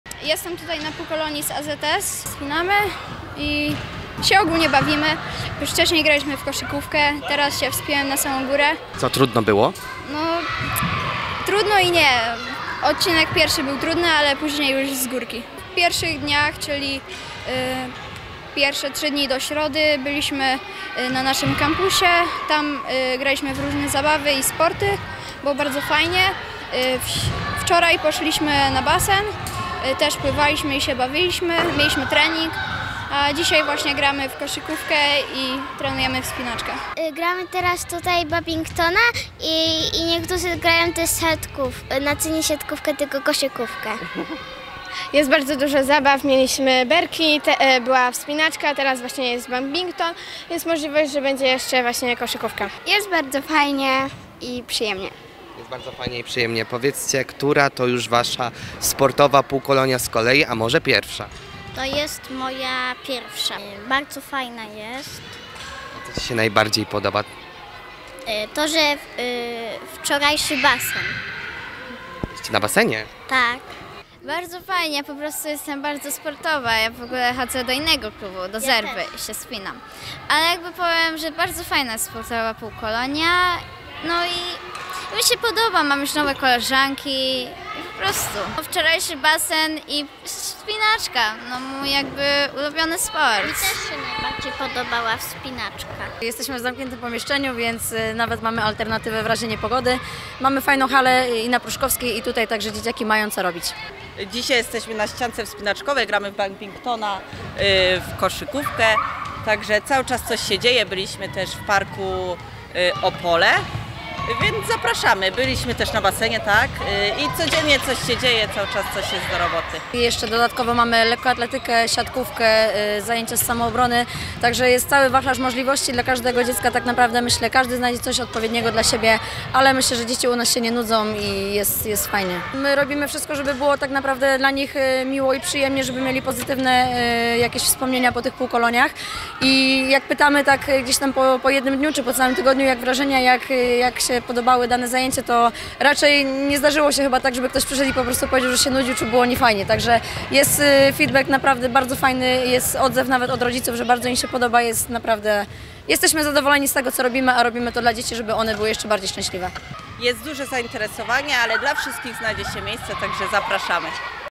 Sportowe wakacje w Opolu - AZS Politechniki Opolskiej zaprasza
Pierwsze wakacyjne turnusy półkolonii AZS4Kids już się zakończyły. Uczestnicy z zainteresowaniem brali udział w licznych zajęciach sportowych a tegoroczna półkolonia jest dodatkowo wzbogacona o  nowe atrakcje, m.in. basen, ścianka wspinaczkowa oraz pobyt w rodzinnym parku rozrywki pod Opolem. Na kolejne turnusy są jeszcze wolne miejsca.

📰Szukasz więcej informacji na temat KULTURA I ROZRYWKA? Sprawdź